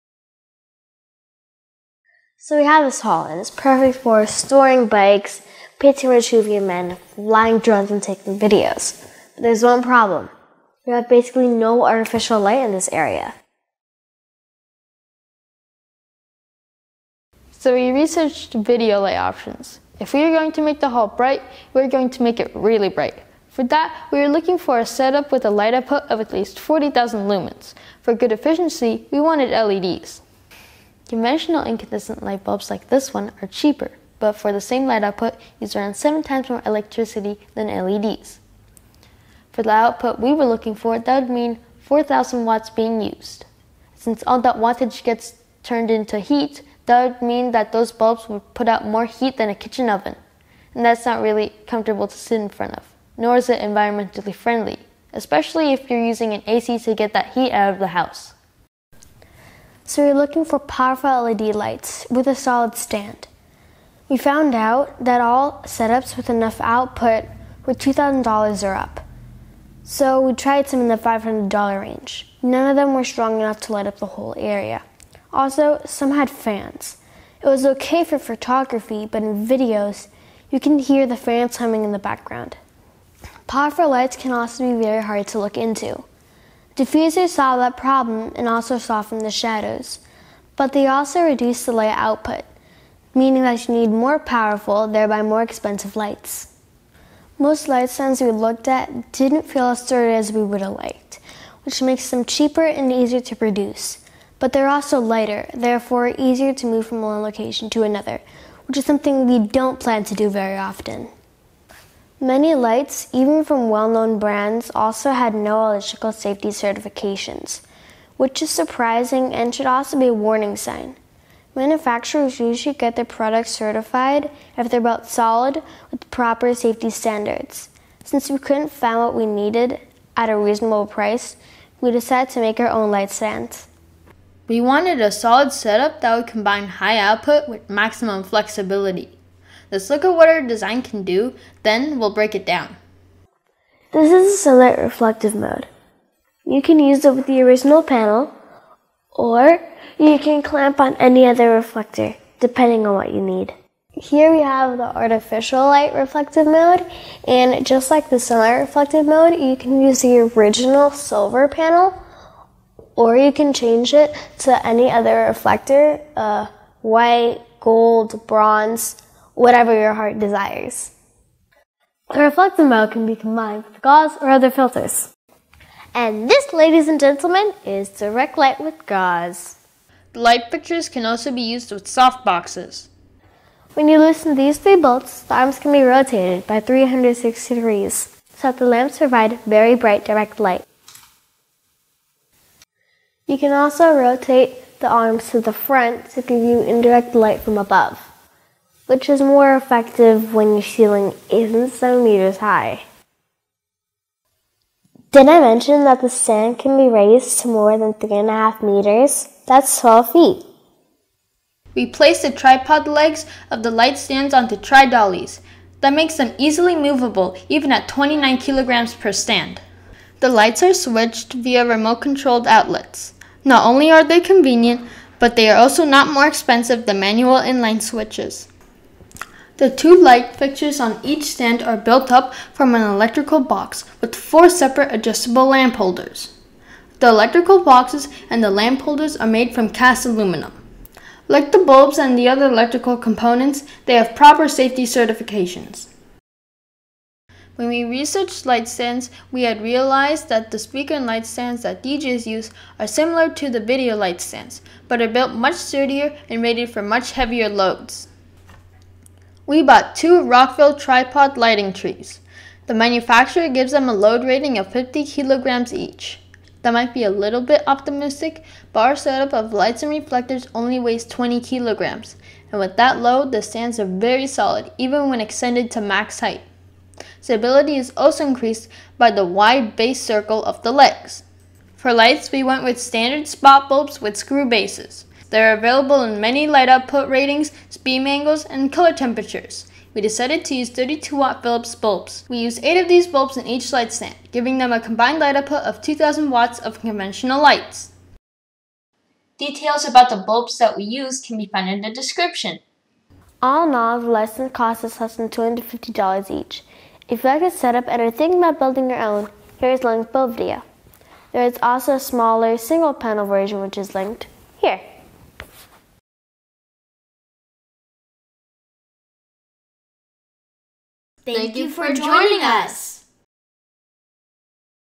Ultimate DIY 2000W LED video light stands - for less than $250
This is the large version of our video light stands with a light output of more than 22,000 lumen each in our configuration.

The light stands can be used with any light bulb with a standard PAR38 socket. We looked around for good deals and bought our 250W-equivalent LED bulbs in a color temperature of 3000K on Amazon from Light Bulbs International (Philips Commercial 32W LED; 2800 lumen; $39.99 per case of 6 bulbs) and a 5000K alternative from Lowes (Sylvania 26W Night Chaser; 2400 lumen; on sale for $8.90/each). There are also photo/video specific PAR38 bulbs with very high CRI values (Color Rendering Index) available, but those are usually really expensive.
The bulbs that we got work perfectly for our use.

All electrical components of the light stands are safety certified.